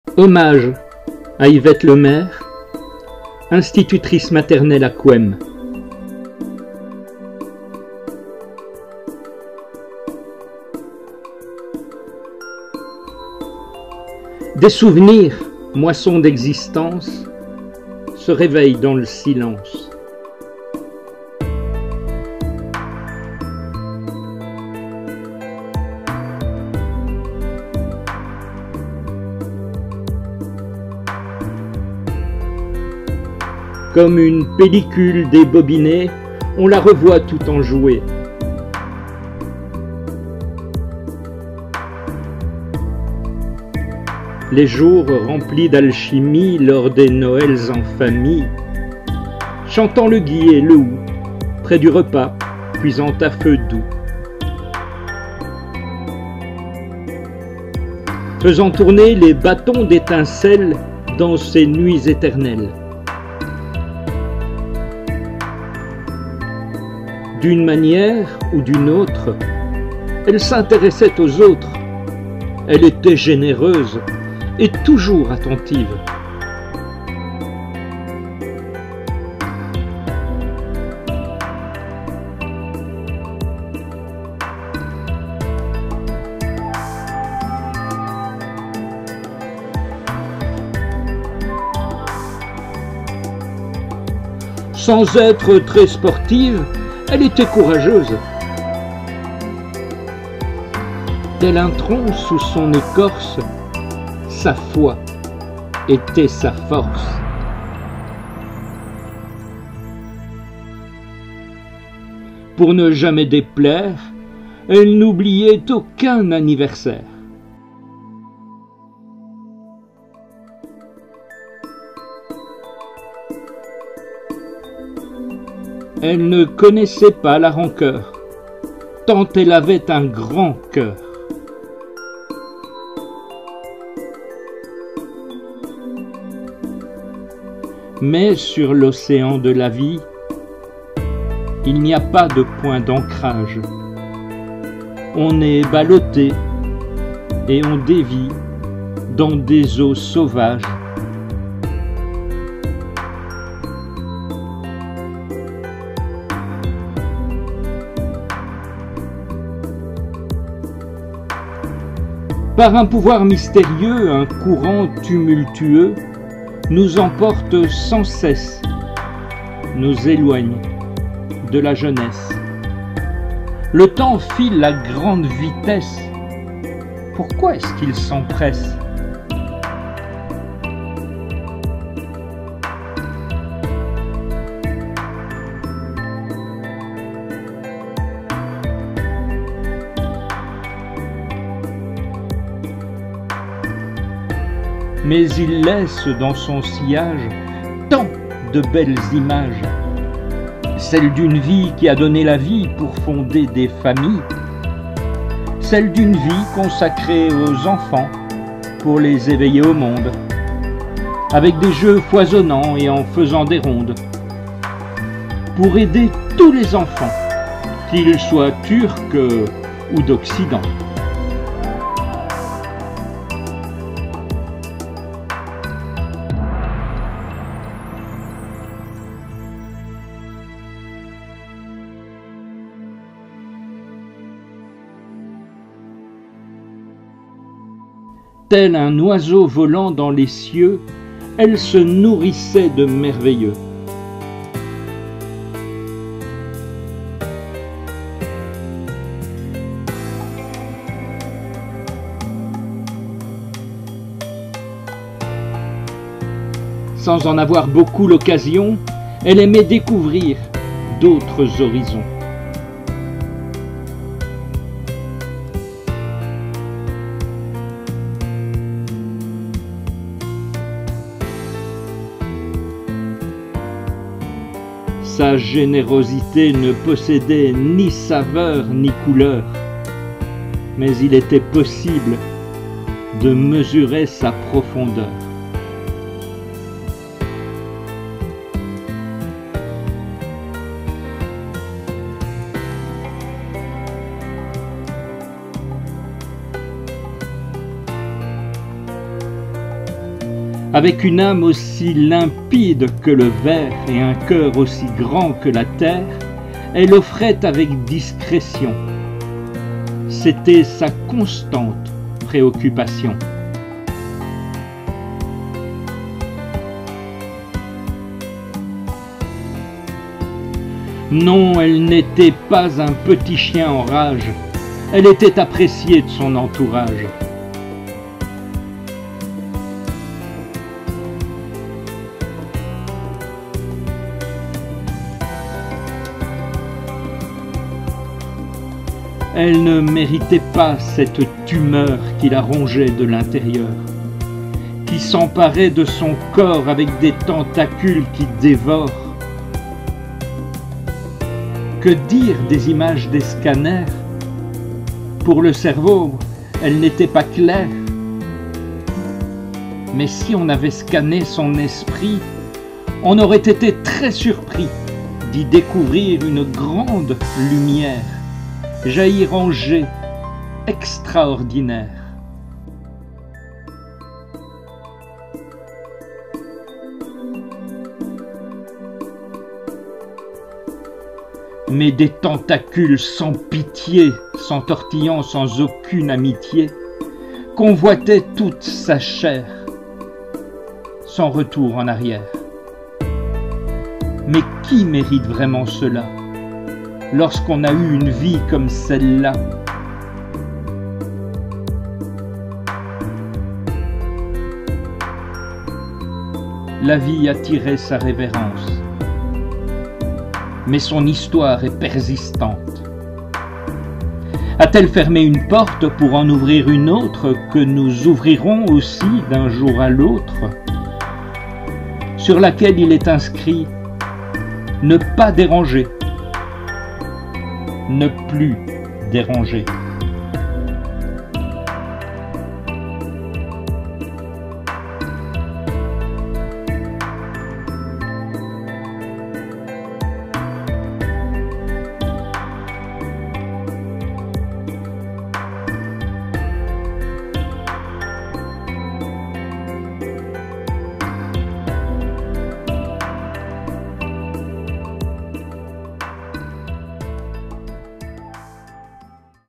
Hommage à Yvette LEMAIRE, aussi connue sous le nom d'Yvette DELSAUT, son nom de femme mariée.
Yvette LEMAIRE fut une institutrice maternelle qui a marqué son époque à Cuesmes, Anderlues et à Quaregnon.